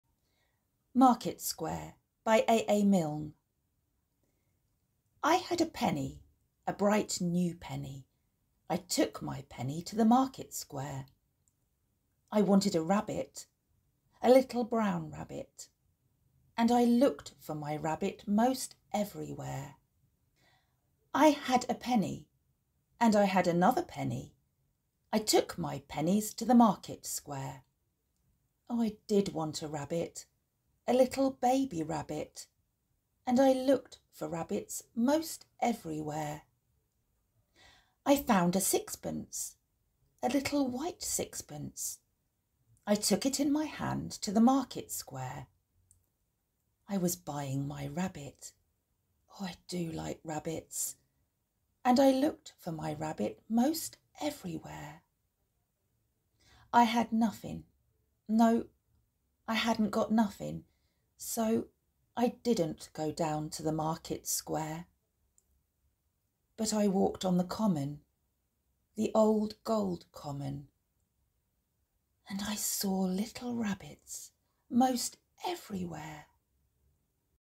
🐇 A.A. Milne Market Square | Easter Poem | Rabbit Poem
Market Square by A.A. Milne is a poem from 'When We Were Very Young'. The collection was first published in 1924. In the poem, Christopher Robin is desperate for a rabbit and keeps visiting the market to by one with his pennies. However, it isn't until the very last stanza that the little boy finds any rabbits...

A.A. Milne was born in London, England, on January 18, 1882. He is best known for his Winnie The Pooh stories and poems. His poetry collections for children: When We Were Very Young (published in 1924) and Now We Are Six (published in 1927) were very popular with children and are still popular with both children and adults all over the world to this day.

I had a penny,
A bright new penny,
I took my penny
To the market square.
I wanted a rabbit,
A little brown rabbit,
And I looked for a rabbit
'Most everywhere.

For I went to the stall where they sold sweet lavender
("Only a penny for a bunch of lavender!").
"Have you got a rabbit, 'cos I don't want lavender?"
But they hadn't got a rabbit, not anywhere there.

And I had another penny,
I took my pennies
I did want a rabbit,
A little baby rabbit,
And I looked for rabbits
'Most everywhere.

And I went to the stall where they sold fresh mackerel
("Now then! Tuppence for a fresh-caught mackerel!").
"Have you got a rabbit, 'cos I don't like mackerel?"

I found a sixpence,
A little white sixpence.
I took it in my hand
I was buying my rabbit
I do like rabbits),
And I looked for my rabbit
'Most everywhere.

So I went to the stall where they sold fine saucepans
("Walk up, walk up, sixpence for a saucepan!").
"Could I have a rabbit, 'cos we've got two saucepans?"

I had nuffin',
No, I hadn't got nuffin',
So I didn't go down
But I walked on the common,
The old-gold common…
And I saw little rabbits
'Most everywhere!

So I'm sorry for the people who sell fine saucepans,
I'm sorry for the people who sell fresh mackerel,
I'm sorry for the people who sell sweet lavender,
'Cos they haven't got a rabbit, not anywhere there!